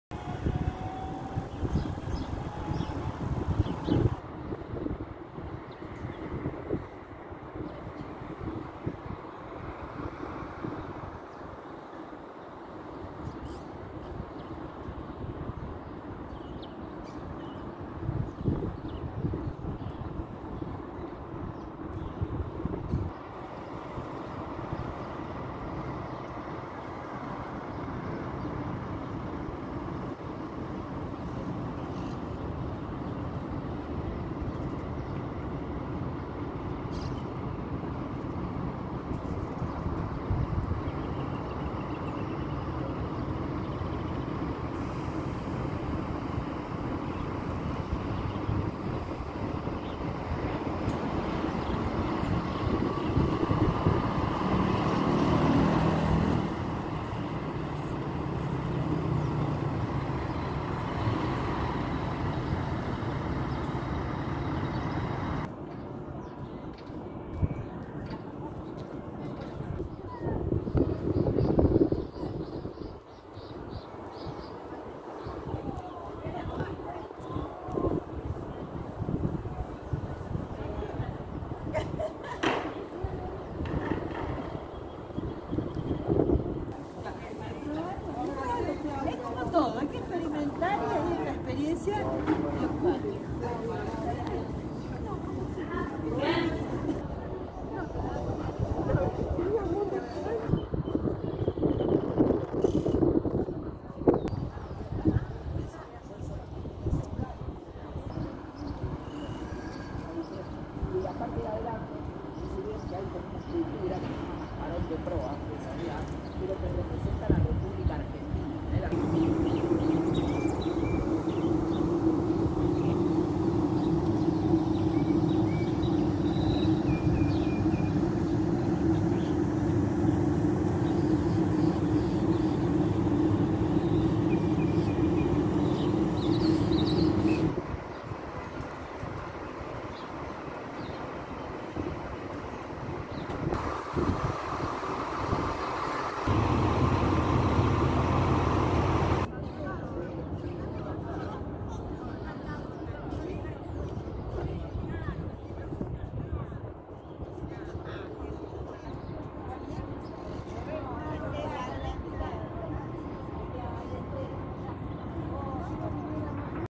Puerto Madero - Buenos Aires
Ab 1990 wurden die Lagerhäuser des 'Holzhafen' in ein modernes Wohnquartier umgewandelt. Unzählige Lokale säumen die Pier, einige Hafenkräne erinnern noch an die frühere Zeit. Das riesige Segelschulschiff Sarmiento der Marine kann besichtigt werden. Die Puente de la Mujer (Brücke der Frau) ist derzeit wegen Renovierung geschlossen.